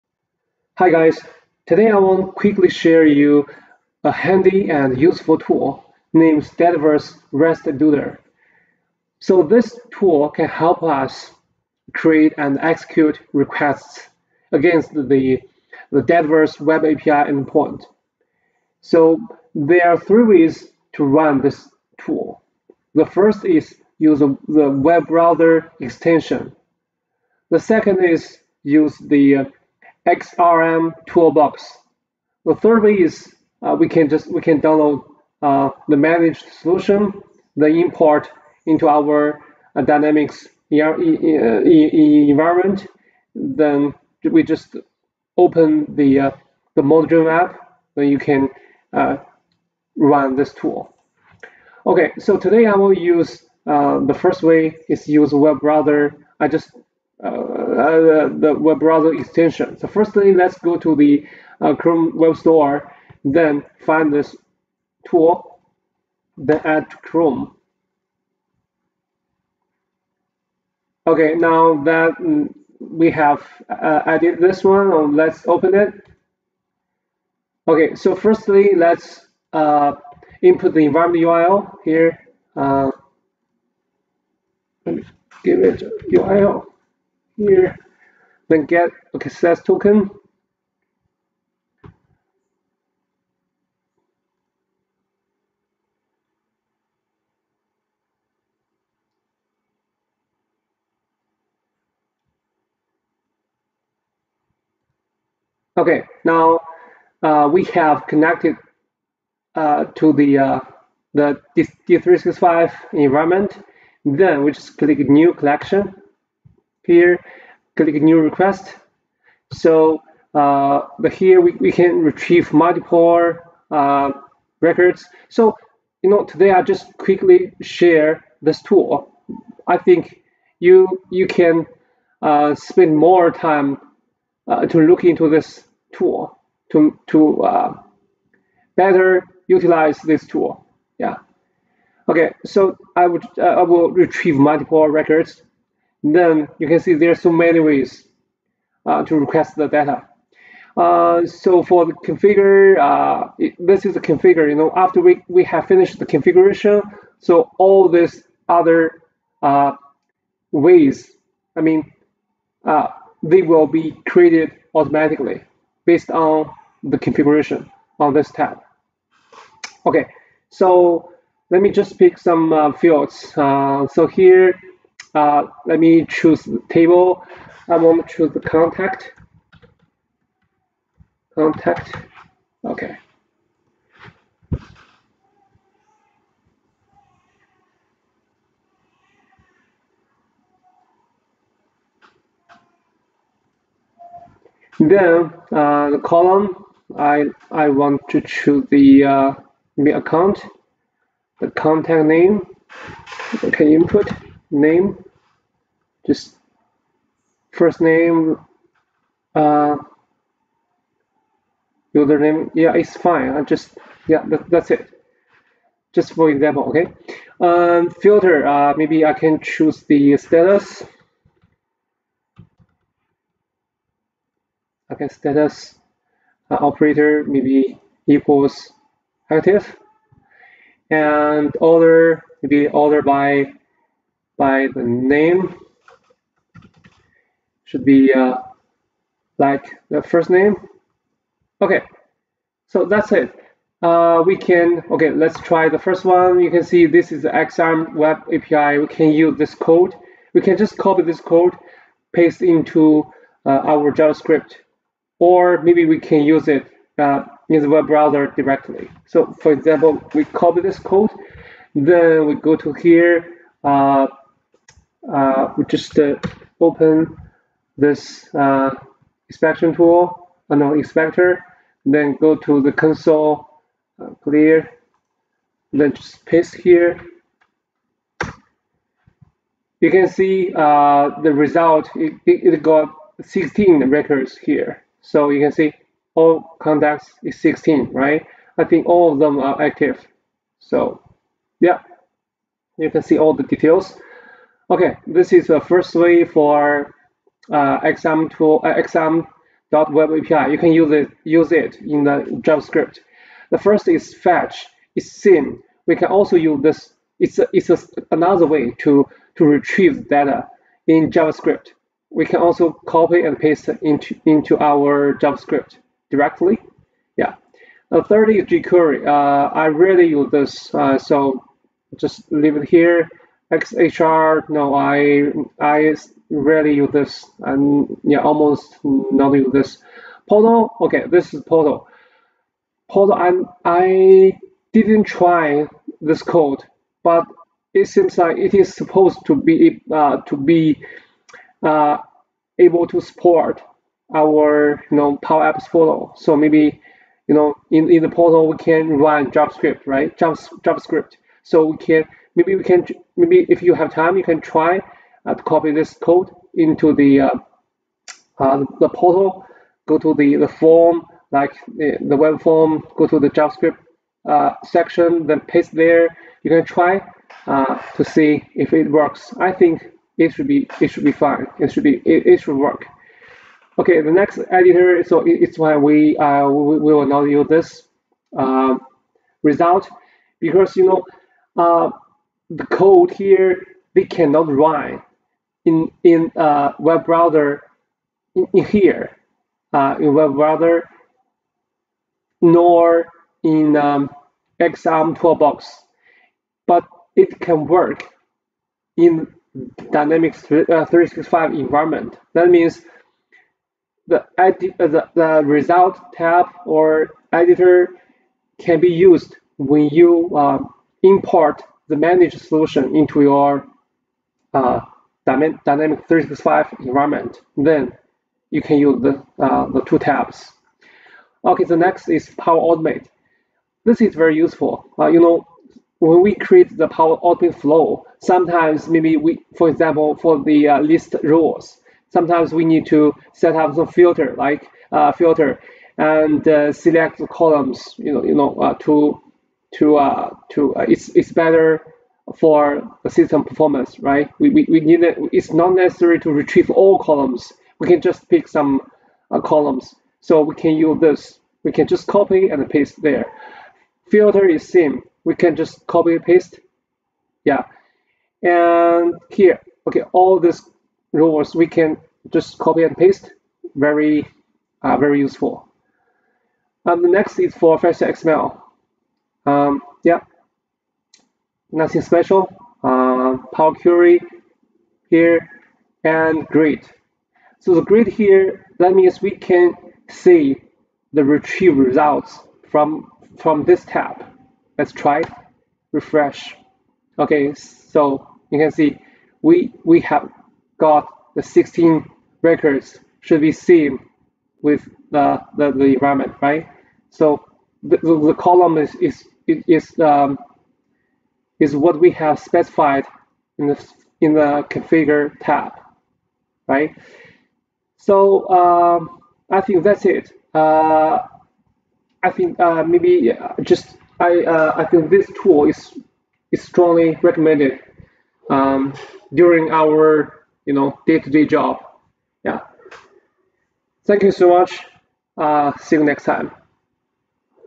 Use Dataverse REST Builder to Create and Execute requests against the Dataverse Web API endpoint
Create and Execute requests against the Dataverse Web API endpoint.
Dataverse REST Builder allows to create and execute requests against the Dataverse Web API endpoint. After obtaining the token from the authentication process (a Dataverse instance is required and a URL must be provided) the tool is loaded and a new collection of requests can be created.
Each request matches a capability of Dataverse, in addition to the standard CRUD it supports specific Dataverse requests like Associate/Disassociate, Execute Workflow and Manage Image and File Data. For each request the tool generates the code for different syntaxes, like Xrm.WebApi (if the code is for a model-driven app), Fetch API, jQuery and Power Apps Portals, some requests generate the code for Power Automate and Power BI.